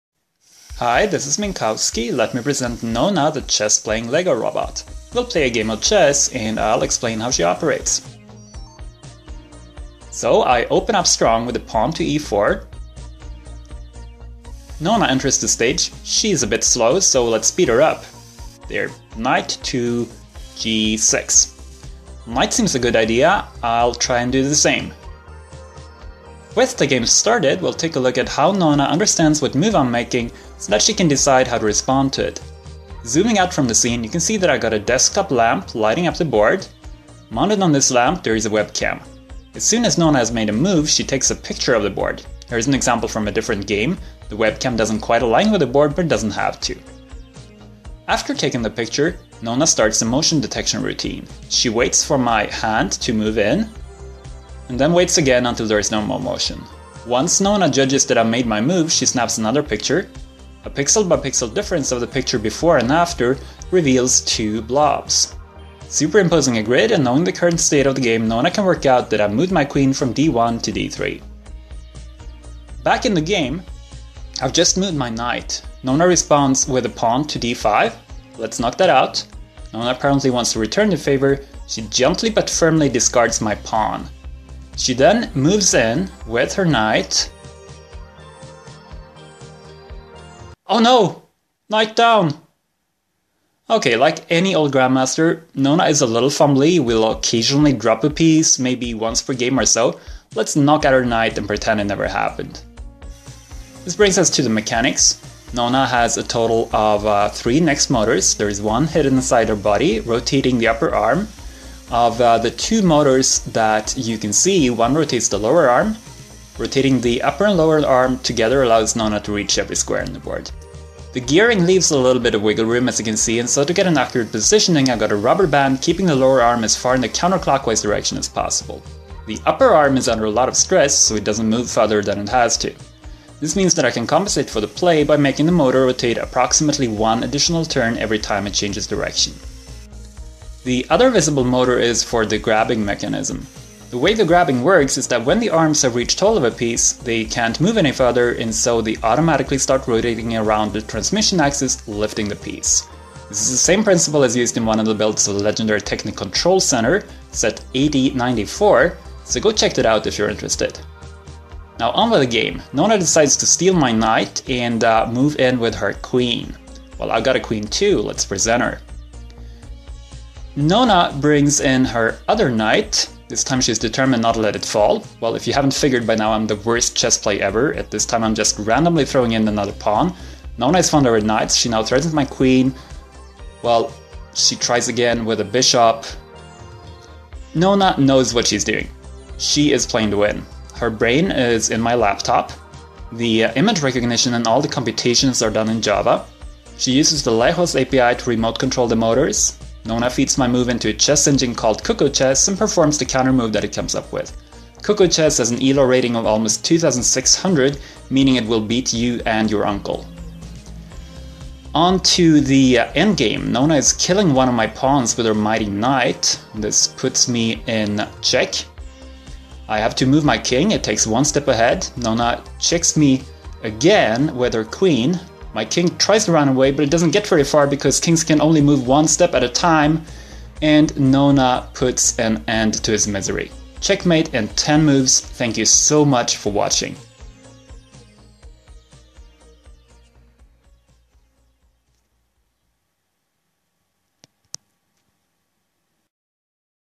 Nona, the Chess-Playing Lego Robot
Named after Nona Gaprindashvili, the first female chess Grandmaster.

You play as white. Nona watches the board through a webcam and processes the image in a laptop to work out your moves. She then talks with a chess engine to find out how to respond. Using her three NXT motors, Nona can grab, lift, and move pieces as she pleases.